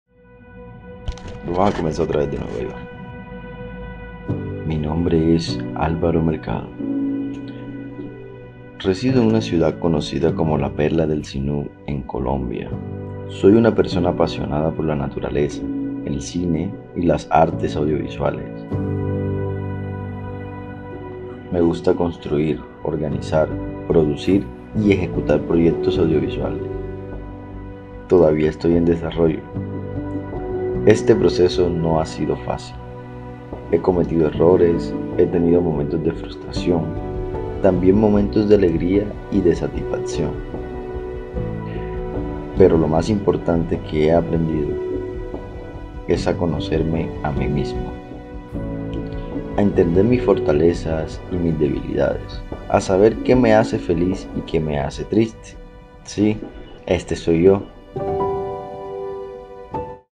Desafío n°1 | Autorretrato 2024 | Alvaro Mercado
En el marco del Festival de Málaga, edición 27.
Los participantes encararon el Primer Desafío, debieron enviar un video minuto, Autorretrato.